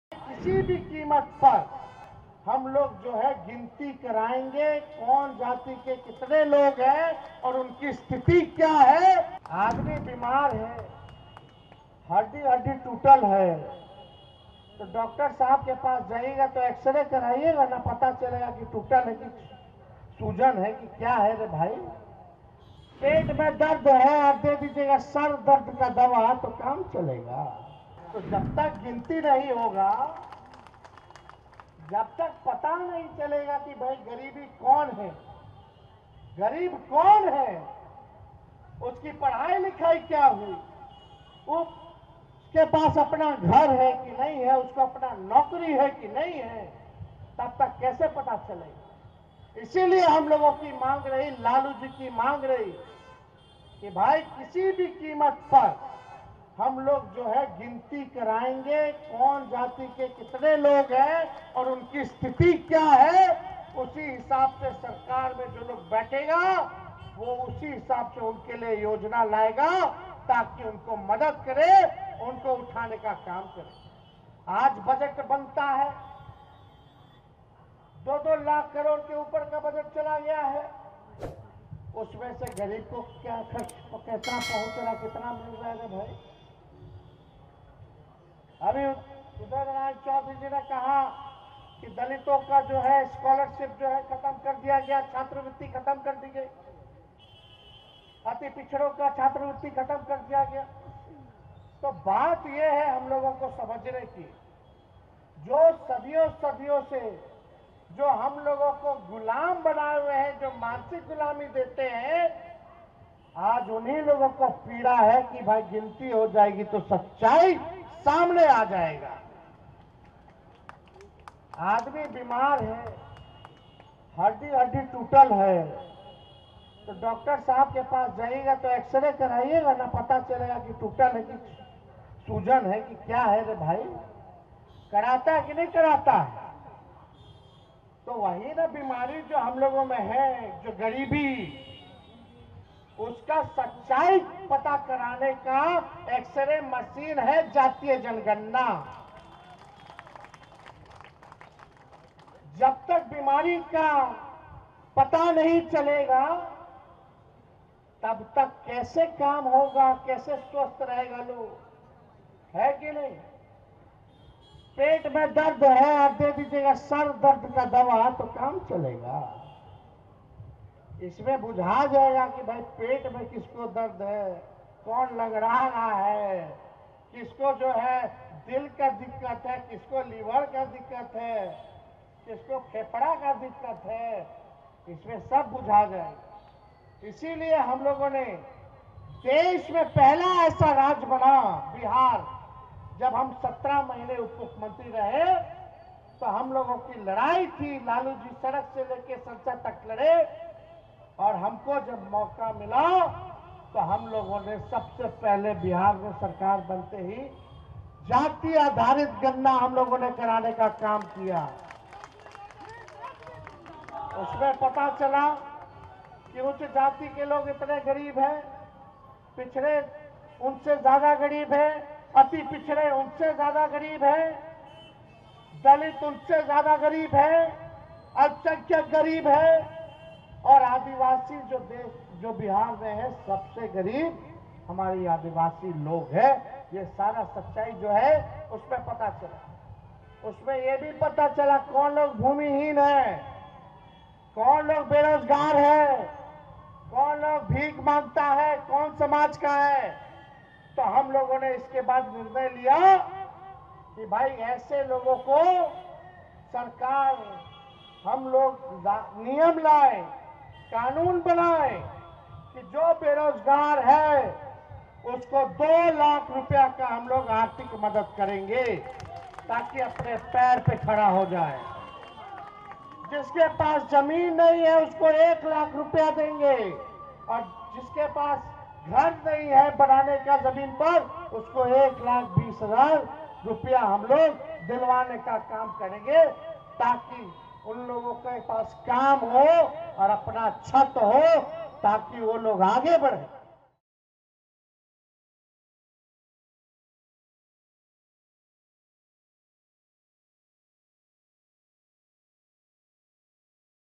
Tejashwi Yadav ने एक रैली को संबोधित करते हुए ऐसे क्या कहा जो बजने लगी तालियां, सुनिए | Hindi News
देश में जातीय जनगणना पर चल रहे सियासी बवाल के बीच Bihar के पूर्व डिप्टी सीएम Tejashwi Yadav ने एक रैली को संबोधित करते हुए NDA Government पर जमकर निशाना साधा है। यही नहीं उन्होंने इस मुद्दे पर Bihar Government पर भी निशाना साधा । सुनिए क्या कुछ कहा...